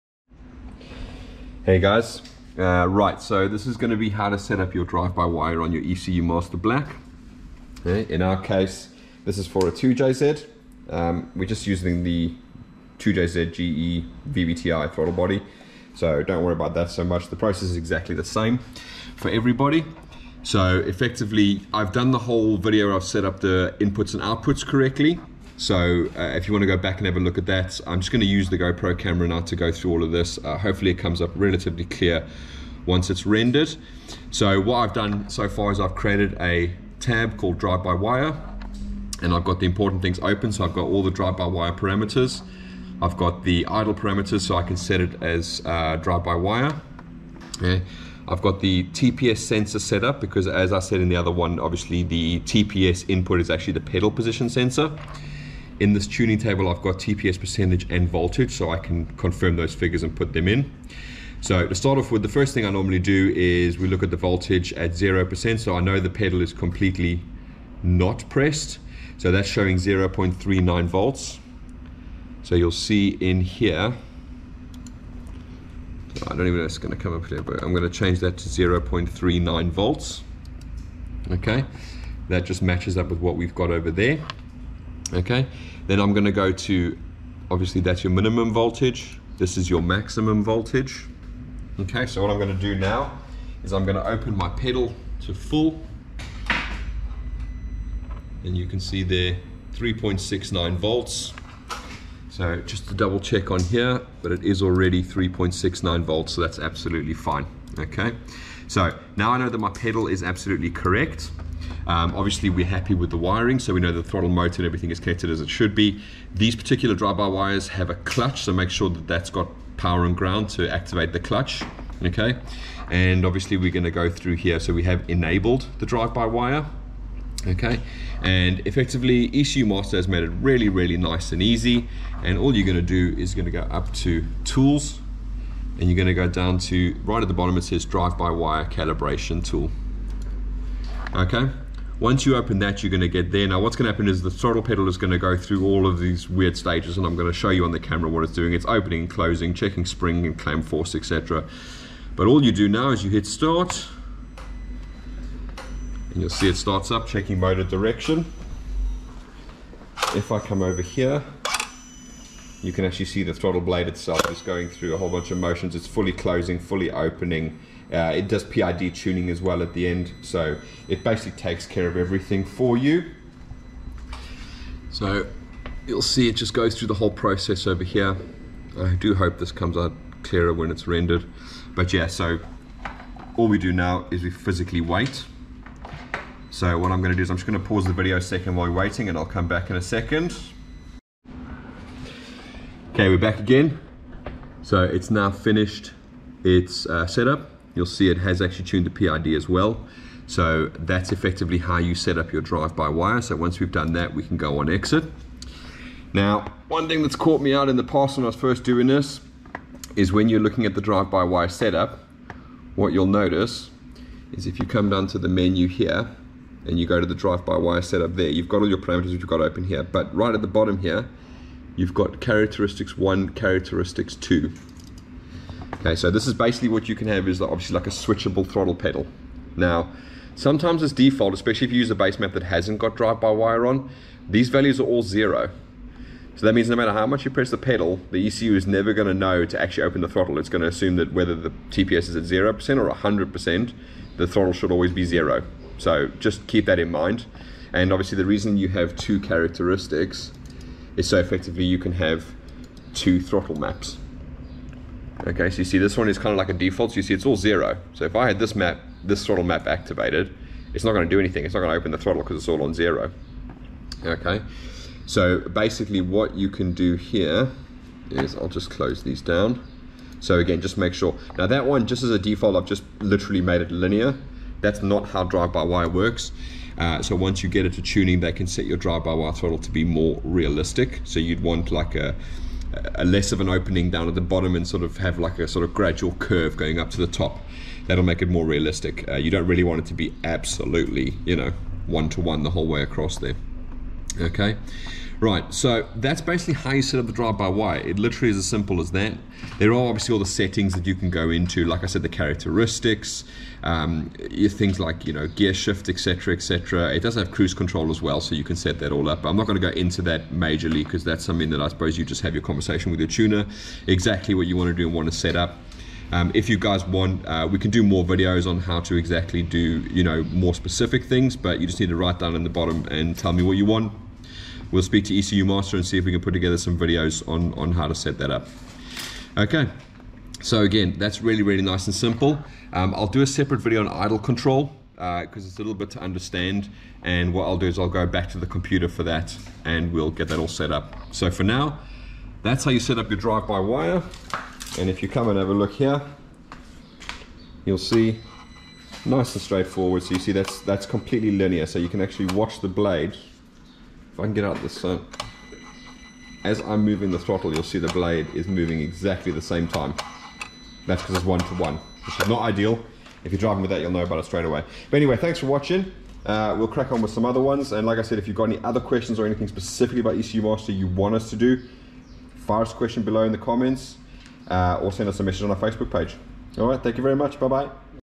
ECUMaster Black How to Setup your Drive By Wire
In the Video we go through how to setup and calibrate your Drive By Wire on your ECUMaster Black ECU